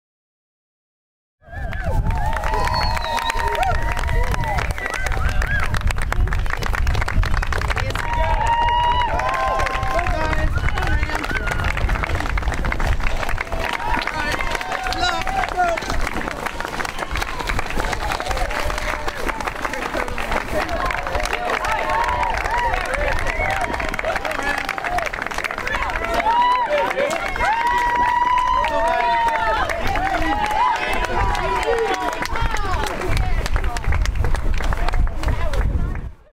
CSU Rams Madness: Fans Sendoff Men's Basketball Team to the NCAA Tournament.
Colorado State University Rams fans gathered at the McGraw Athletic Center to sendoff the Men's Basketball team to Lexington, KY for the first round of the 2013 NCAA Tournament.